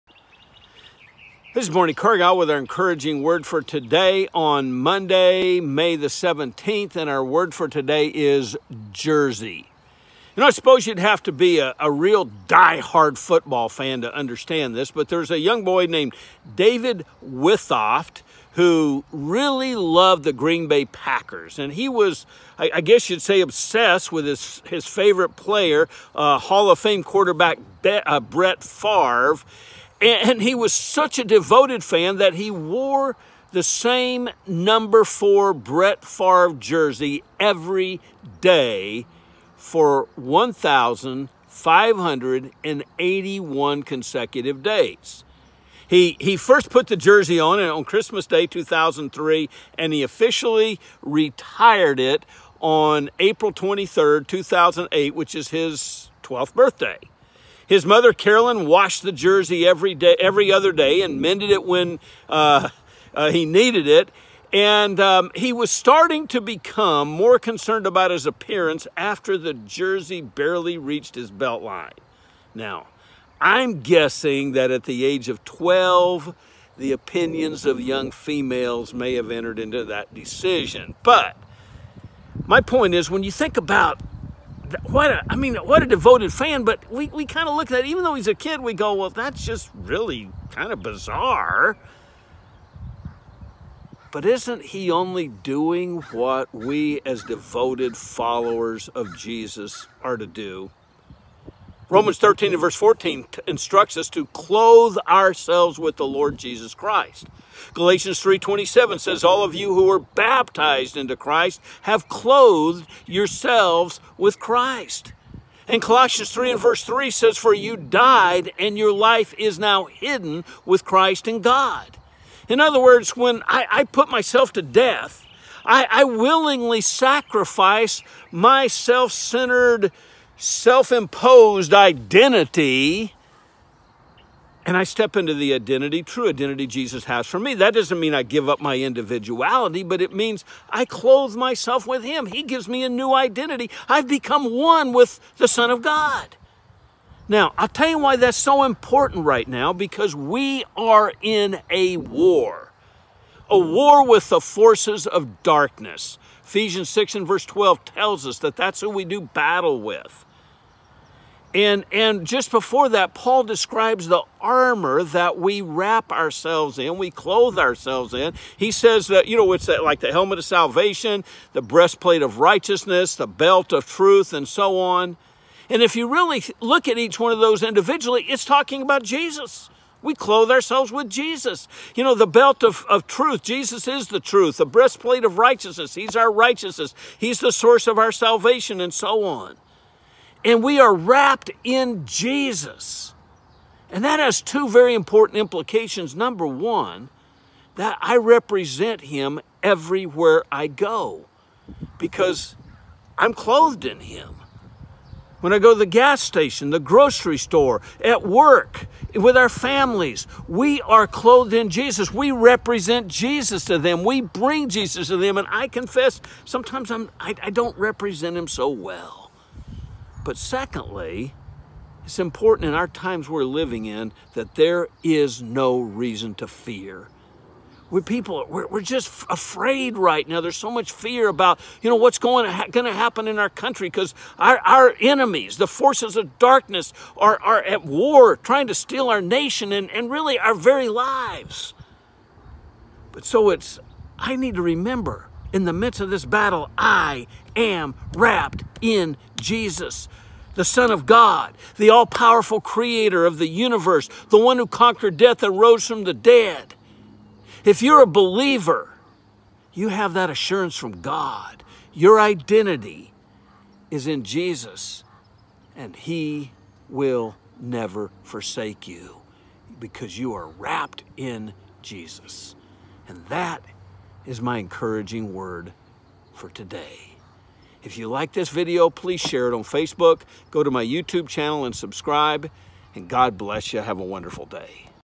CLOTHED WITH BRETT FAVRE
The strange story of a young boy who wore the same football jersey for four years is a living example of how we need to clothe ourselves in Jesus. His armor equips us with power to face the spiritual forces of darkness.